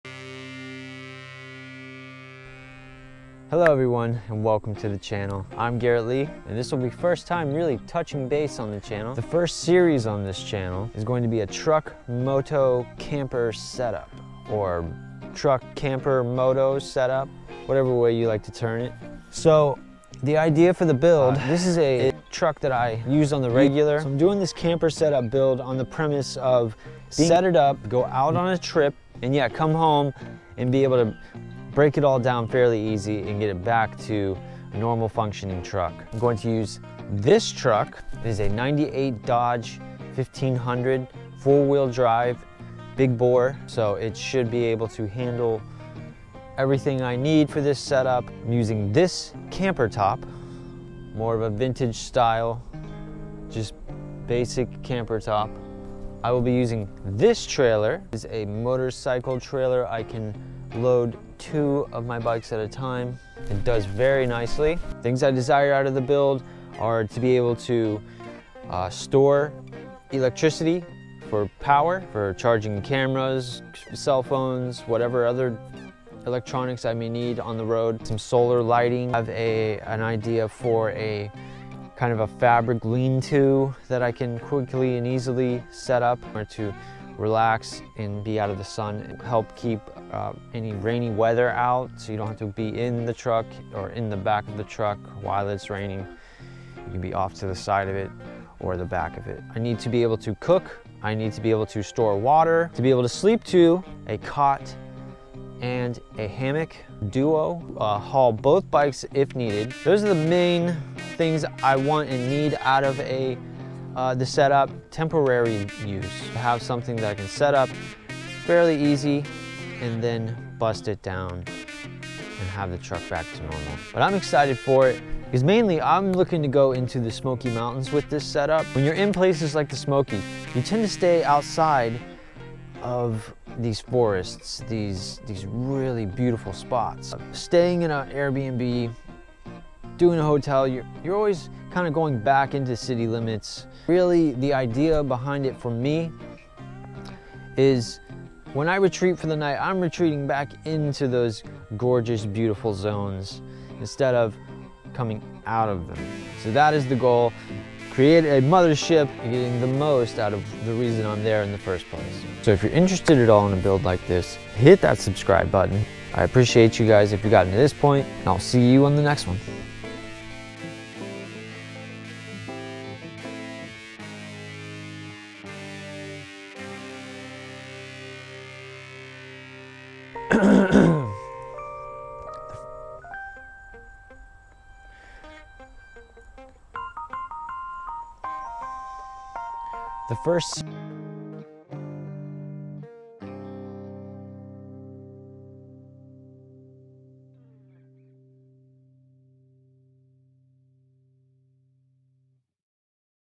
Truck Camper Moto | Project
This is the first video in a series of a truck camper Moto setup videos I’ll be doing. I have a basic idea of what I want from the setup. All the fine details and decisions will be something that evolves as I work through it. The basic needs are to be able to have some type of electricity/power, water storage, be able to cook, good storage space, sleep two, space for two motorcycles, and have a Lean two.